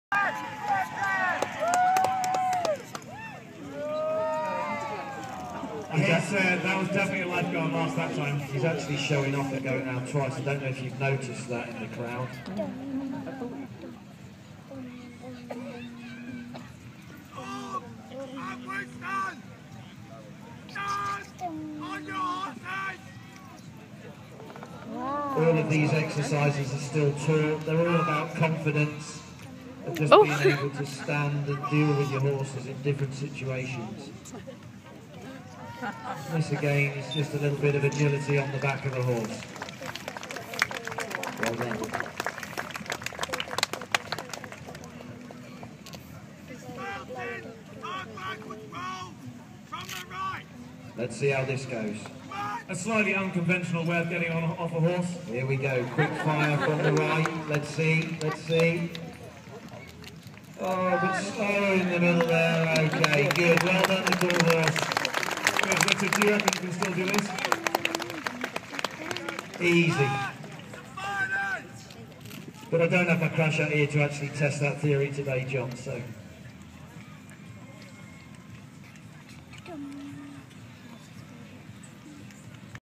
Skills on a horse!
Gymnastics on a horse!
Household cavalry, blues and royals, lifeguards!